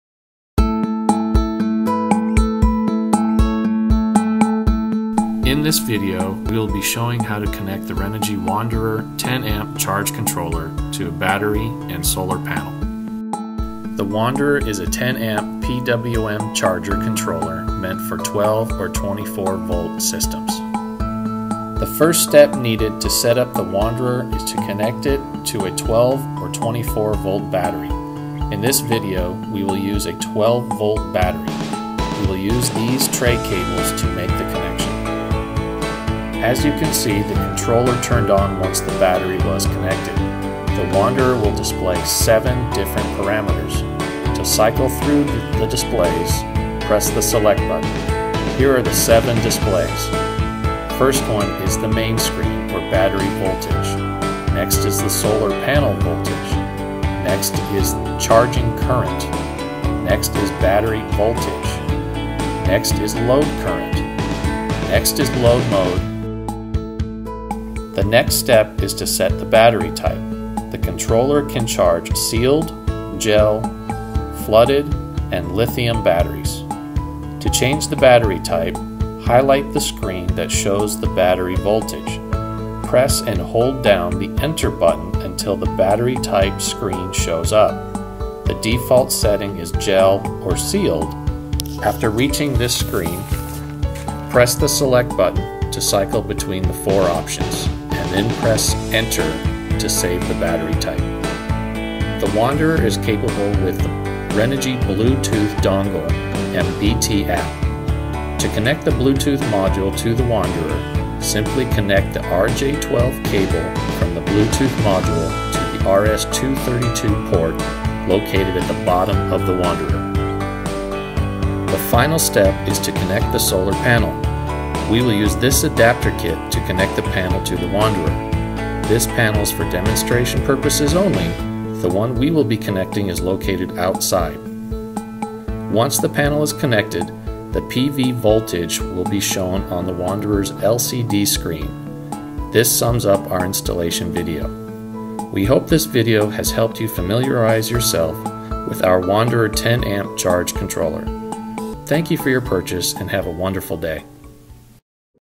【RENOGY】PWMチャージコントローラーWND10A
型番：RNG-CTRL-WND10

12V、24Vバッテリーを自動認識で充電できます。
夜間照明システムをタイマー設定することができます。
USB5V 2A端子、負荷端子付き
バッテリータイプ(Sealed、Gel、Flooded、LFPの設定が可能です。
Renogy BT-1Bluetoothモジュールを通じてパラメーターの設定も可能です。

●以下のサイトよりご購入可能です。
Amazonサイト：